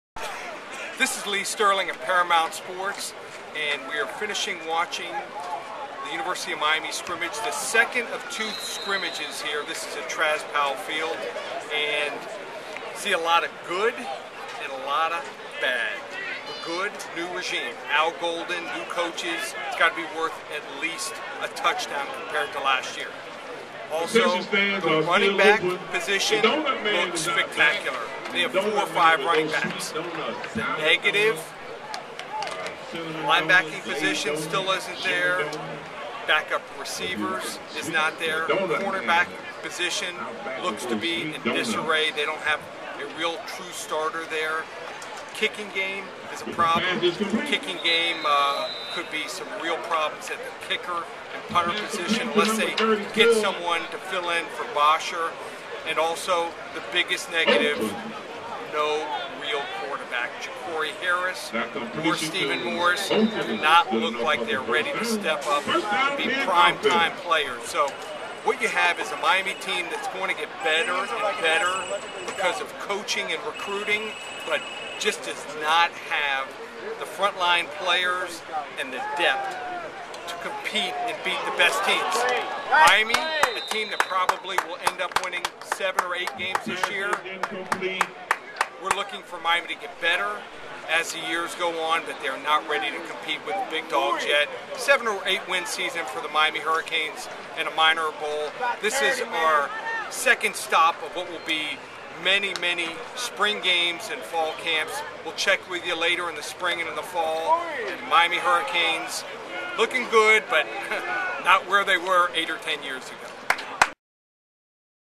Miami Hurricanes Football 2011 Spring Game
Lee Sterling of breaks down the 2011 Miami Hurricanes Spring Football game.